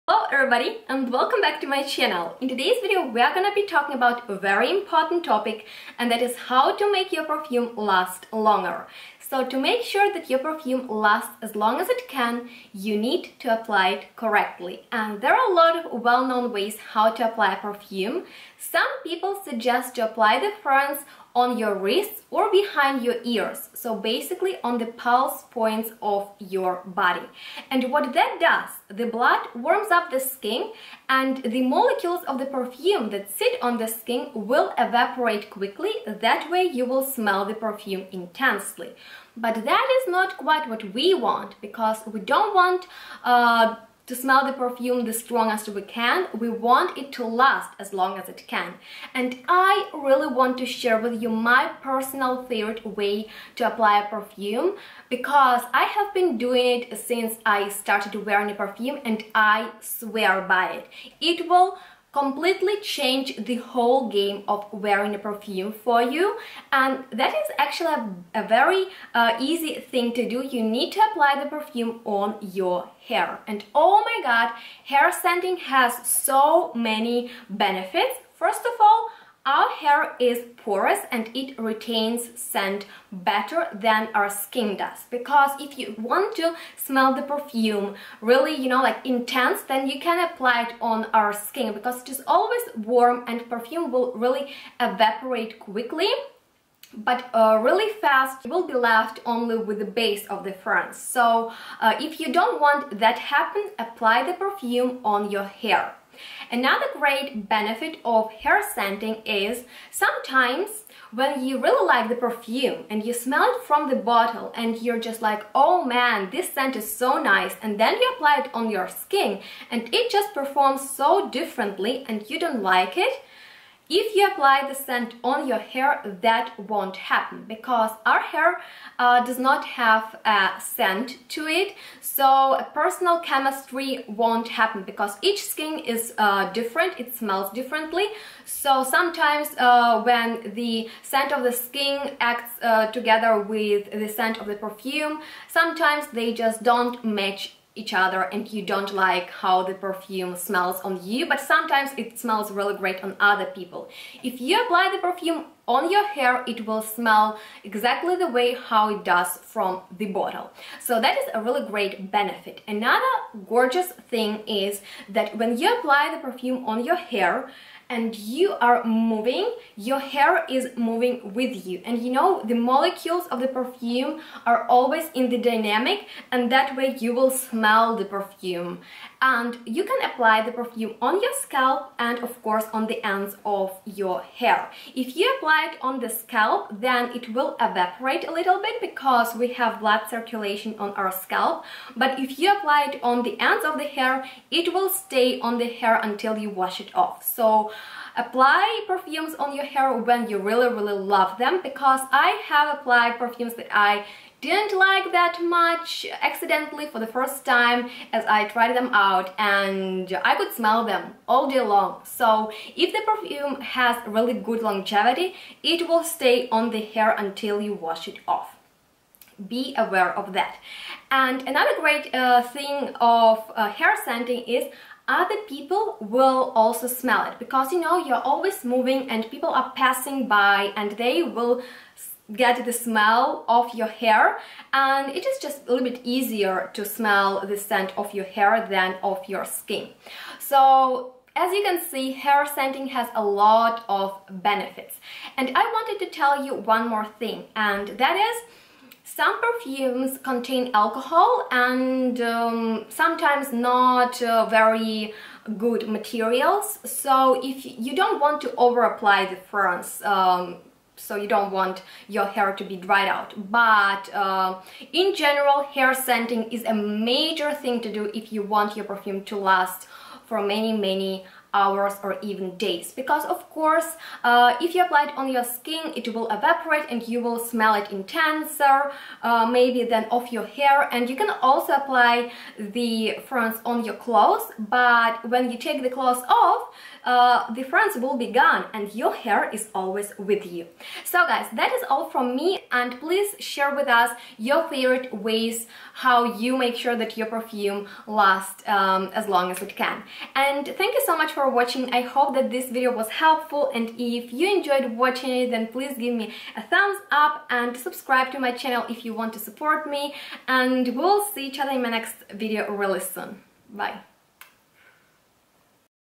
HOW TO MAKE YOUR PERFUME LAST LONGER? HAIR SCENTING! | Tommelise
HELLO EVERYBODY! In this video I share with you benefits of the hair scenting and how you can prolong the wear of your fragrance

xoxo Yana